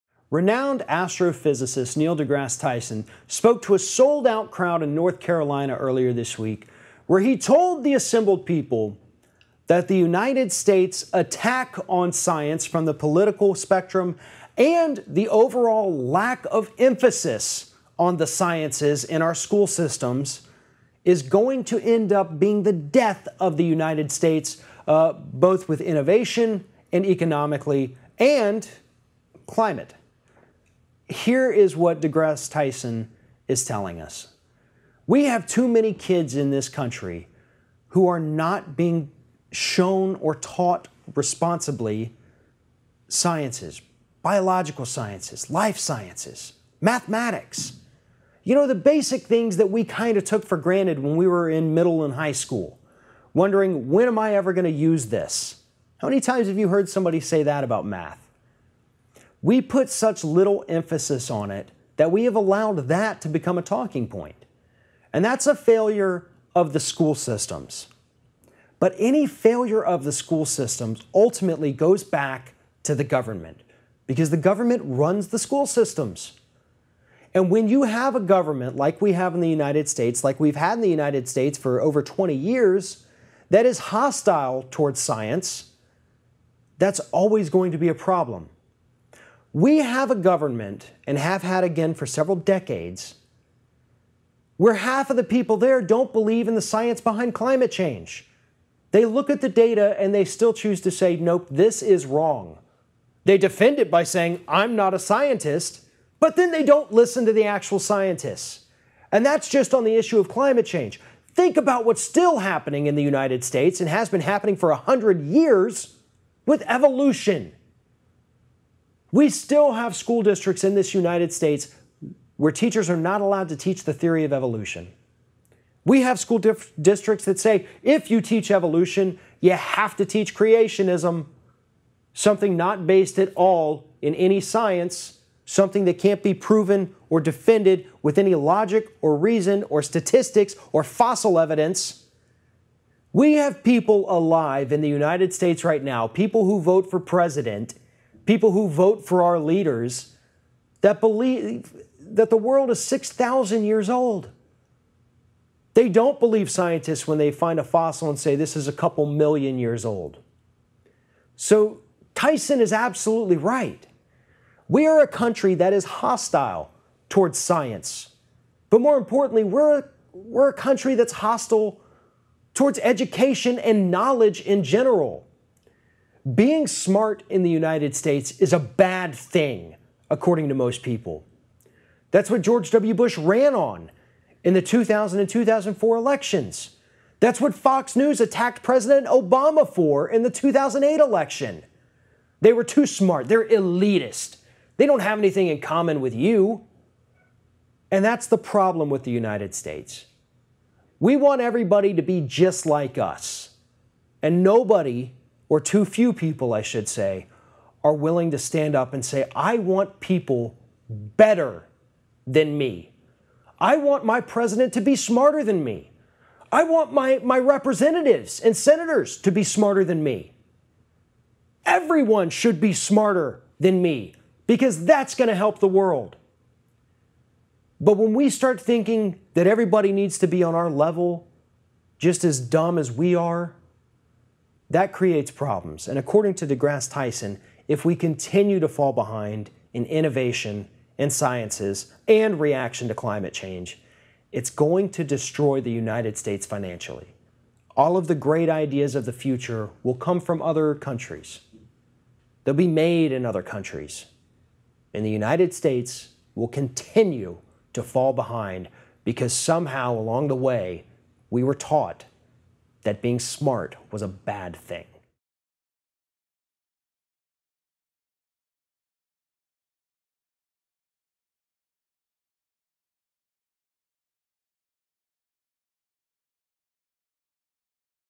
Neil deGrasse Tyson: America's Lack Of Scientific Knowledge Is Dangerous
Neil deGrasse Tyson, one of the most famous and outspoken scientists in the United States, told a sold out crowd in North Carolina that the lack of emphasis on the sciences, as well as the political assault on science, is creating a dangerous epidemic in the U.S. Ring of Fire’s Farron Cousins discusses this.